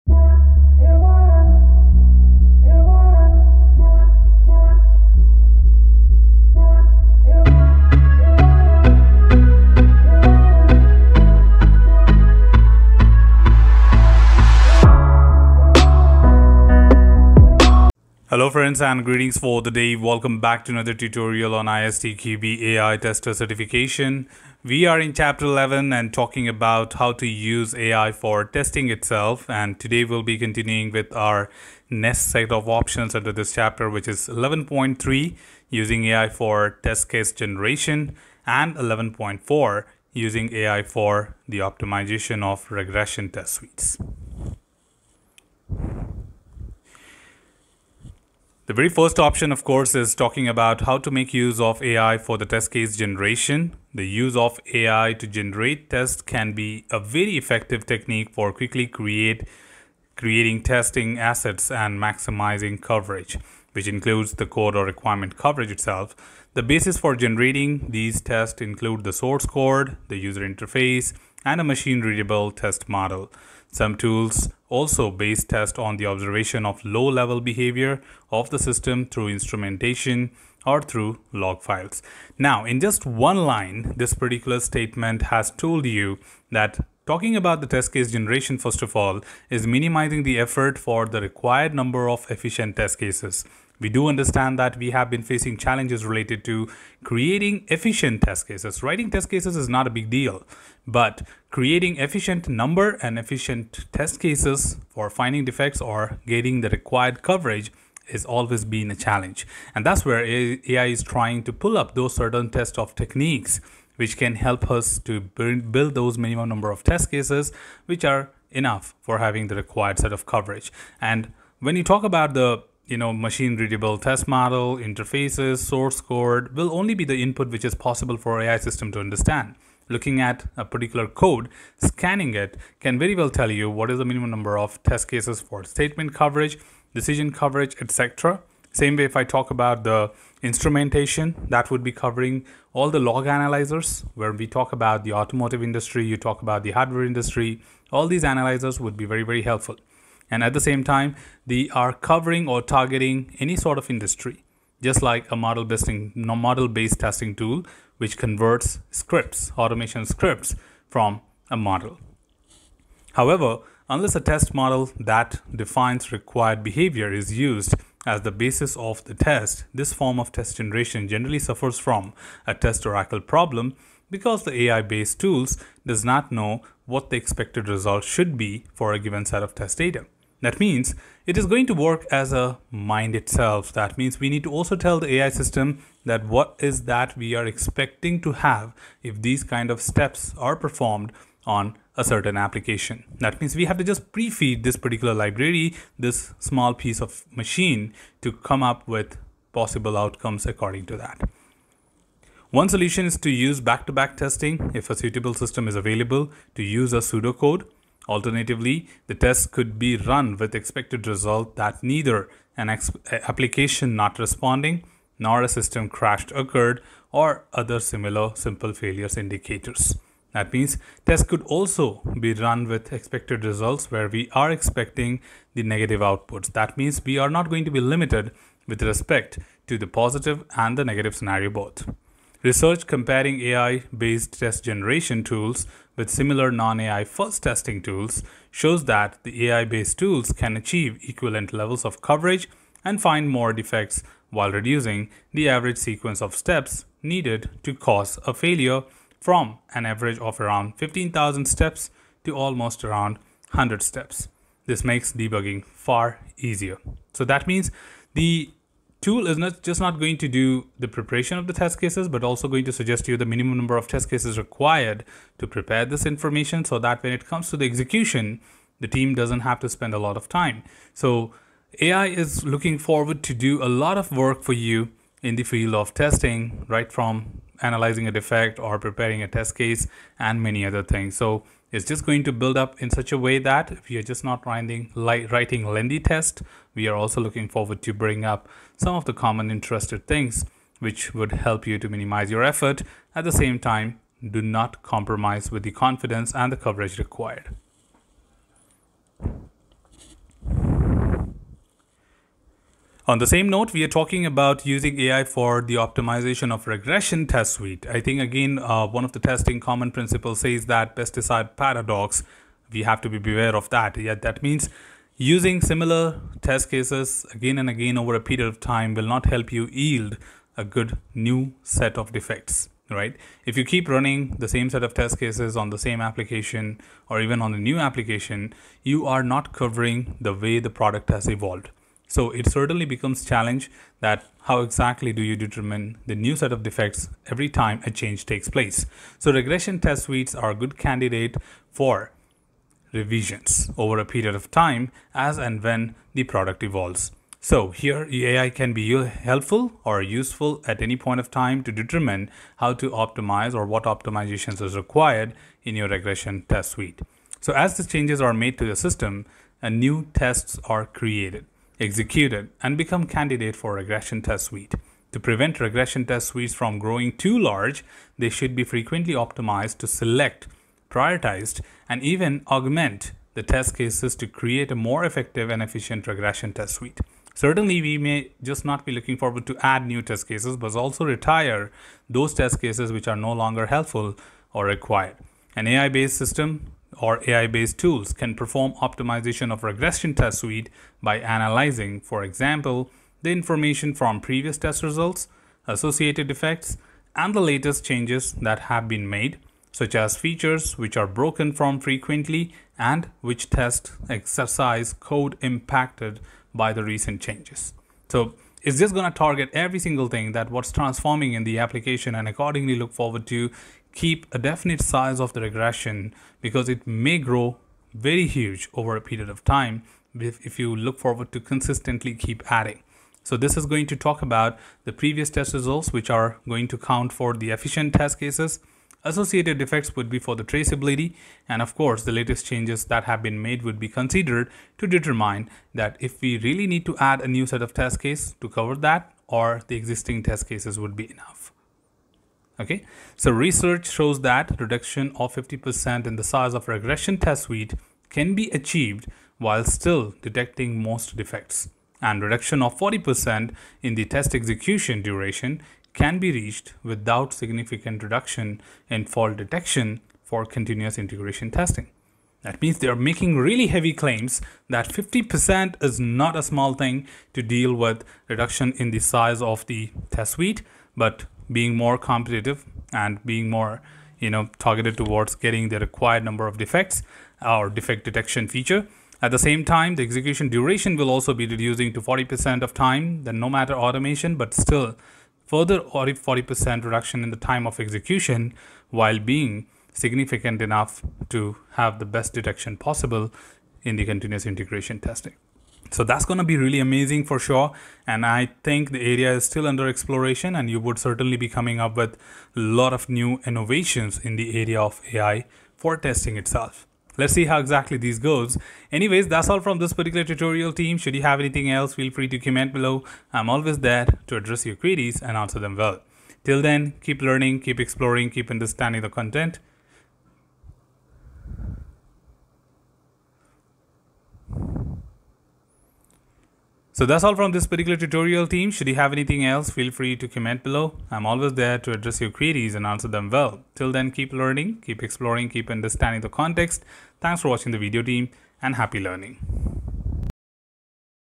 ISTQB AI Tester | Test Case Preparation | Optimizing Regression Test Suite | AI Tutorials
Hello Friends,

This tutorial will drive individuals about the AI for Testing of AI about ISTQB AI Tester Certification of ISTQB Examination.

This series of tutorials will help individuals to prepare for the ISTQB Specialist AI Tester exam. This series consists of ISTQB Syllabus discussion, understanding, tips, tricks, and time management about the exam by Neeraj Kumar Singh.

AI for Testing,
Using AI for Test Case Preparation,
Using AI for Regression Test Suite,
Using AI for Test Activities,